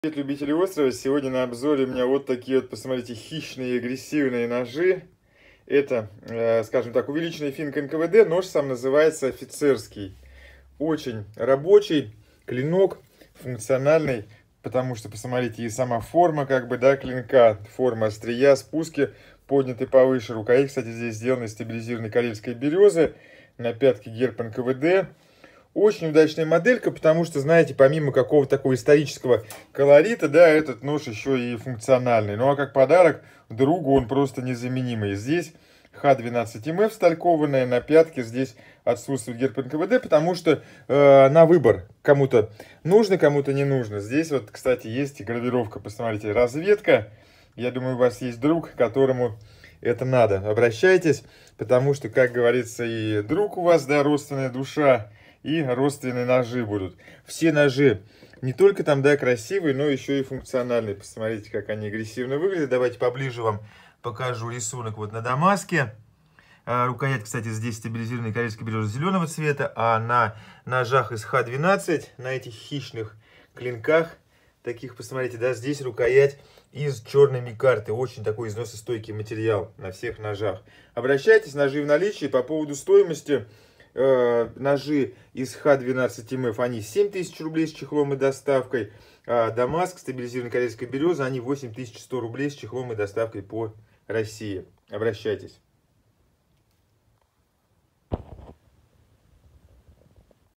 Эти ножи всем хороши
Нож «ОФИЦЕРСКИЙ». Красивый и функциональный👍Для своего гардероба и подарка один из лучших вариантов✅А чтоб похвалиться-то вообще🔝ГРАВИРОВКА В ПОДАРОК🎁Фото этих ножей посмотрите в моем телеграмм